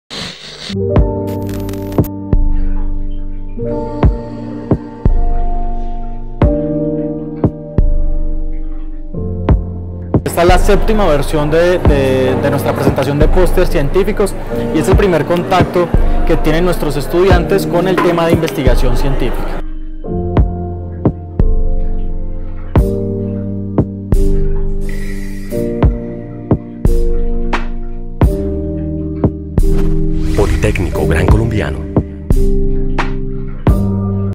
Pósteres Cientificos
En la 7ma versión de posters científicos, un espacio donde nuestros estudiantes tuvieron la oportunidad de exponer sus trabajos que realizaron a lo largo del semestre.